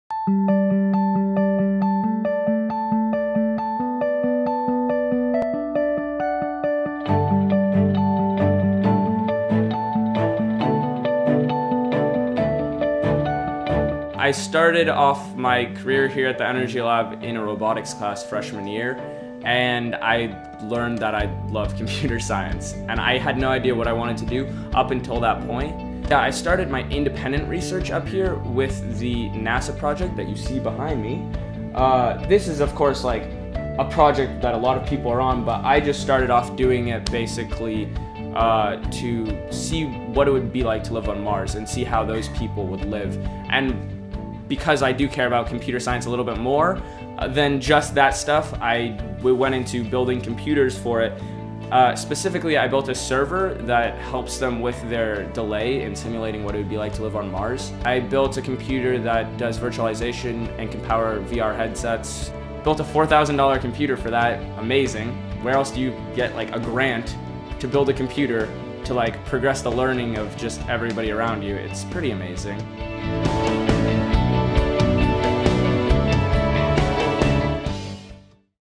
Building a Super Computer: Interviews with Current HPA Students
HPA student Zen Simone '16 talks about his independent research program learning about computer science.